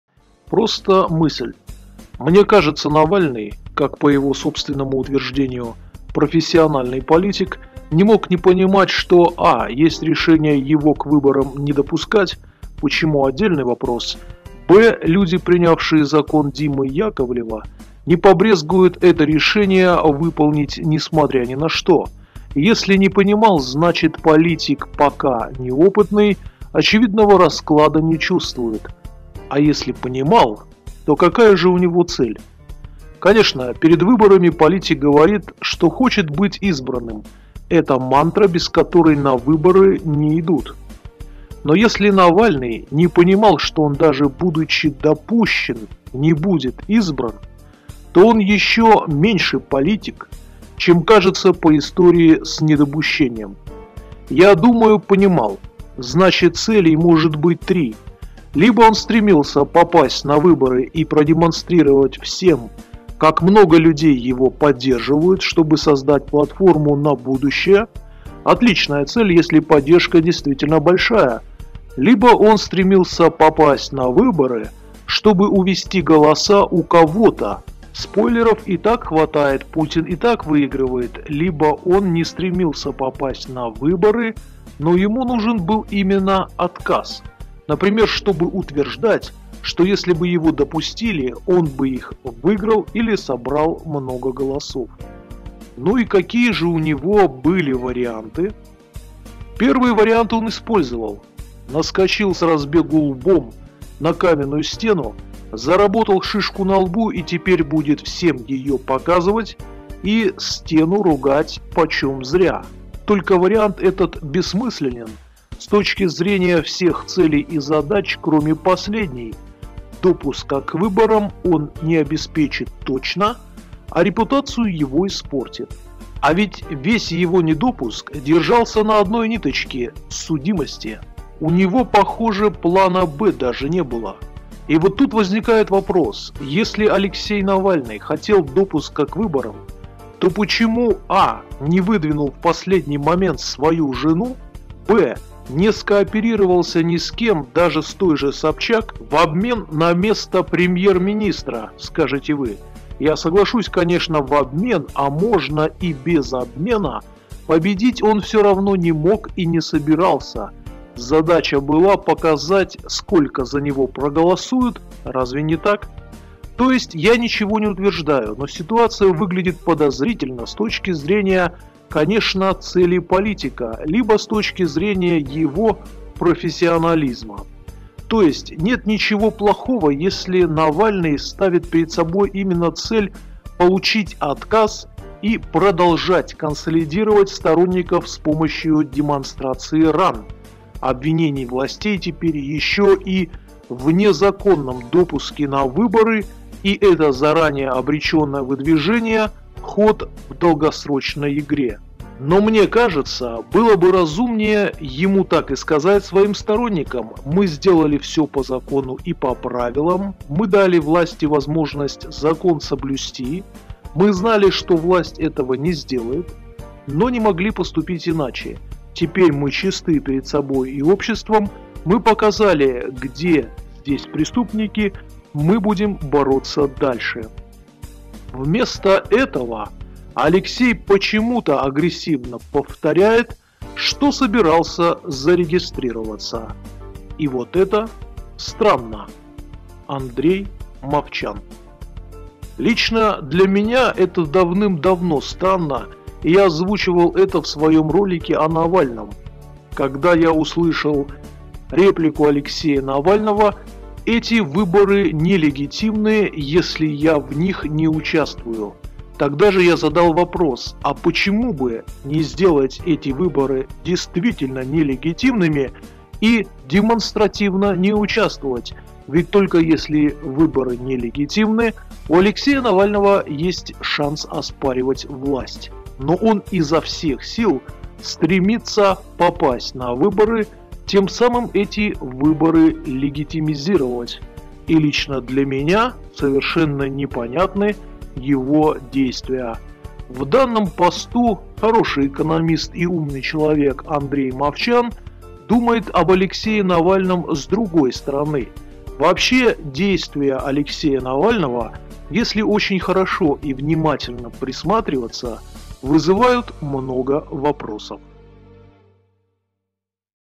⁉️ Что-то подозрительное в Навальном. Андрей Мовчан
У экономиста и публициста Андрея Мовчана появились вопросы о Навальном.